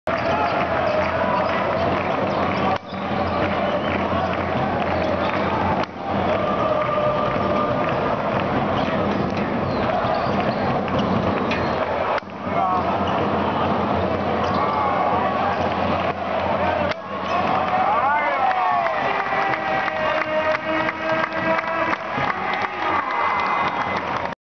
Rebote y canasta Taylor
Gran rebote ofensivo de Taylor que anota a aro pasado